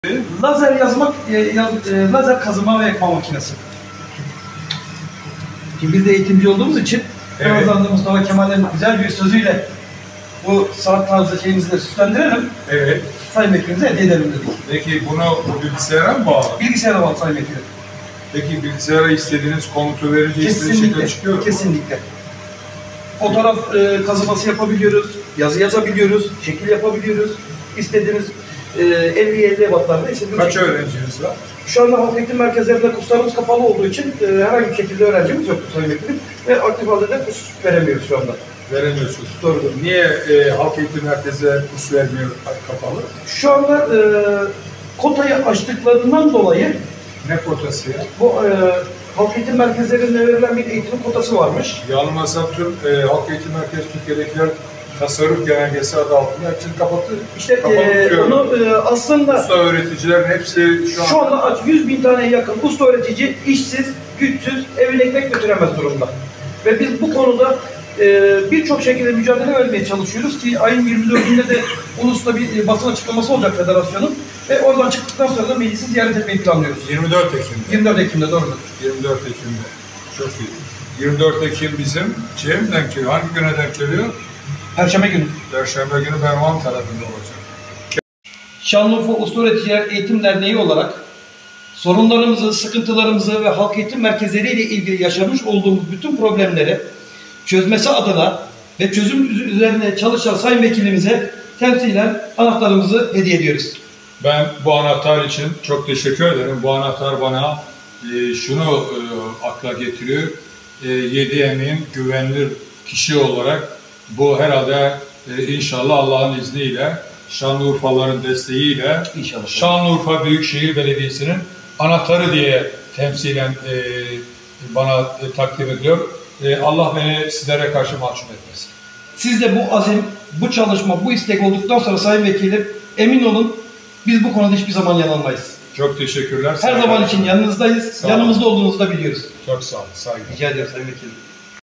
Usta Öğreticileri Mağdur Etmeyin!
Şanlıurfa Usta Öğreticiler Eğitim Derneği’ni ziyaret ettik.

Usta öğreticilerin sorunlarının çözümü noktasında, konunun takipçisi olacağım.

Usta öğreticilerin mağduriyeti artık son bulsun!